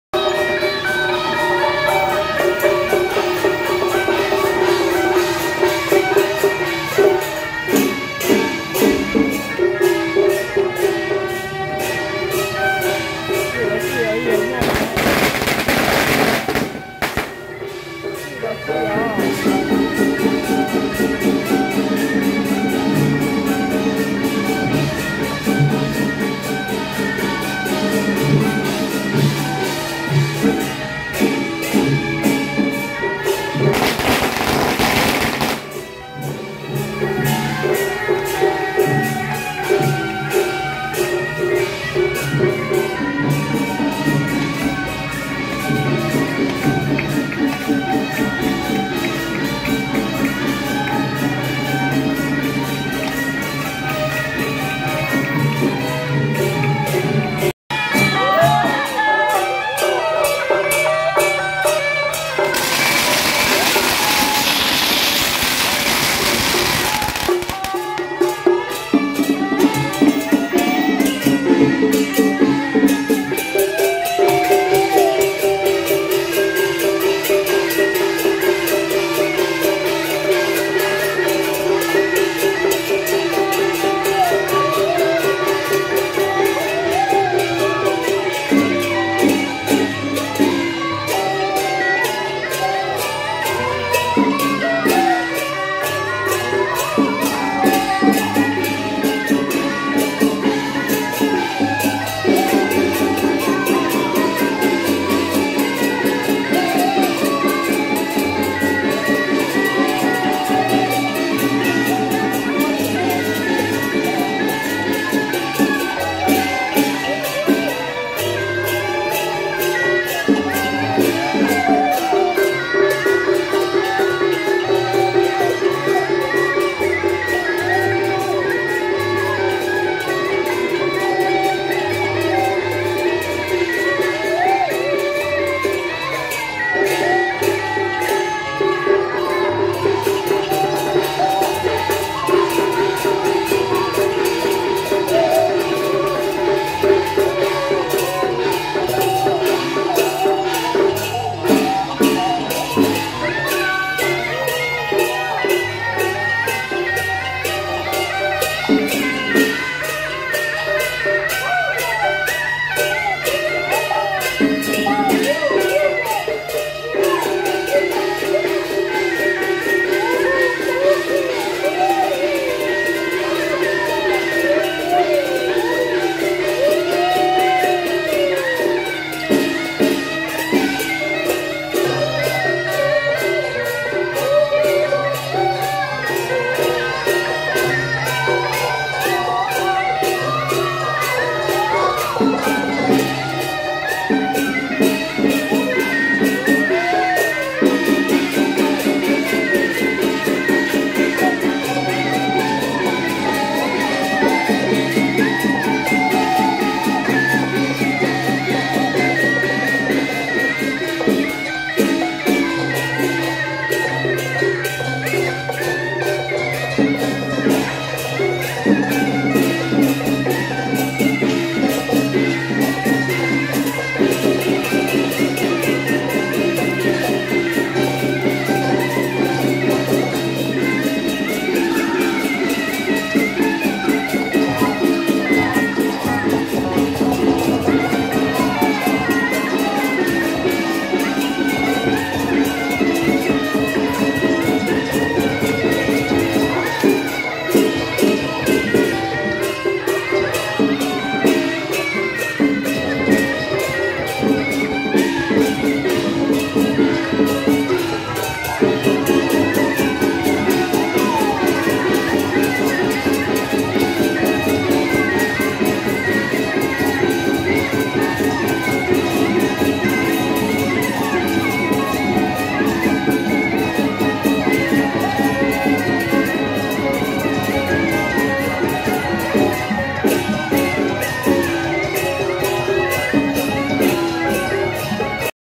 【士林神農宮-安明童、安明瑞、福德正神】~111士林神農宮恭迎天上聖母遶境
台北集斌堂-外江＆士林神農宮-安明童、安明瑞、福德正神 (0:00)過境 士林奏天宮 (0:58)入廟